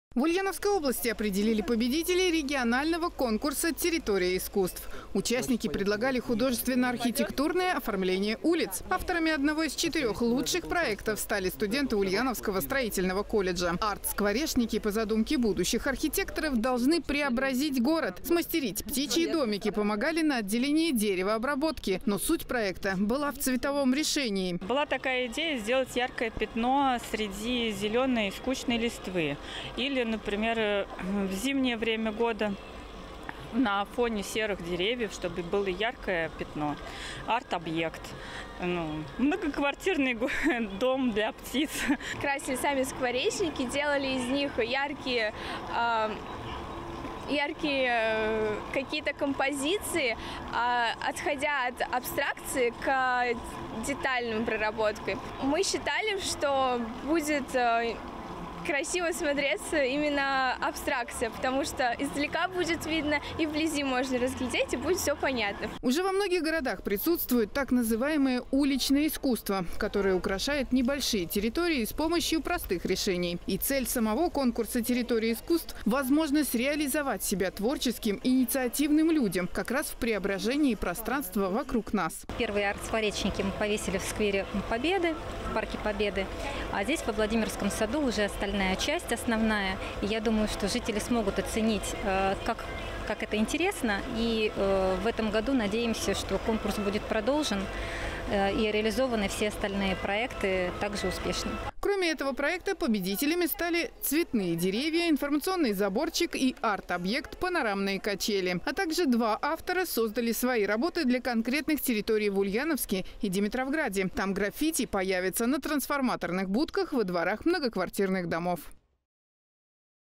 «Территория искусств»
В Ульяновской области определили  победителей регионального конкурса «Территория искусств».